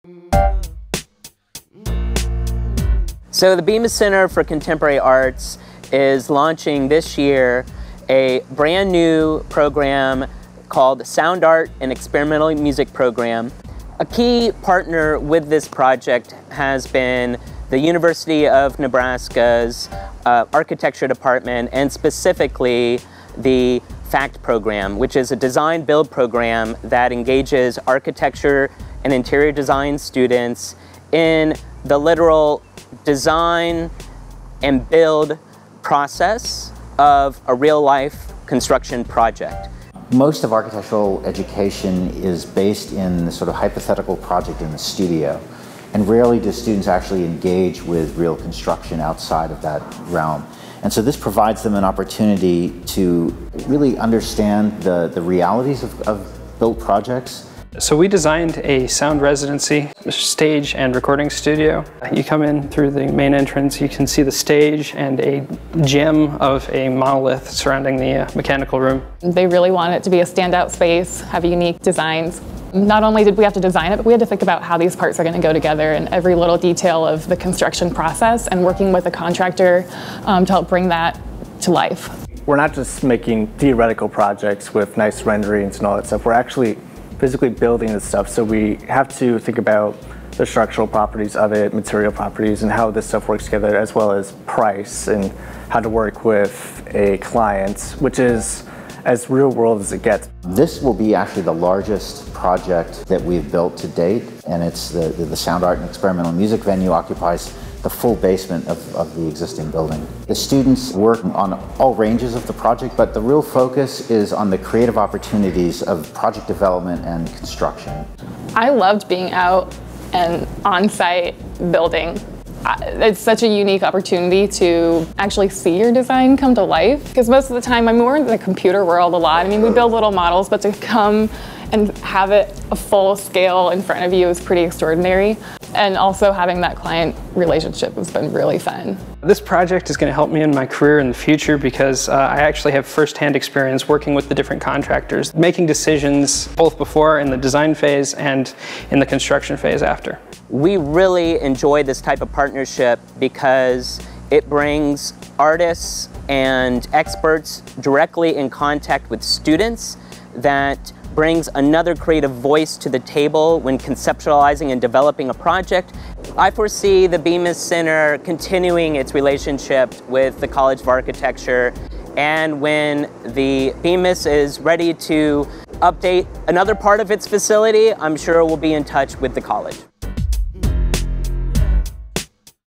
Architecture Students Design Build for Bemis
Students in the University of Nebraska College of Architecture work to design and build a new space in Omaha's Bemis Center for Contemporary Arts.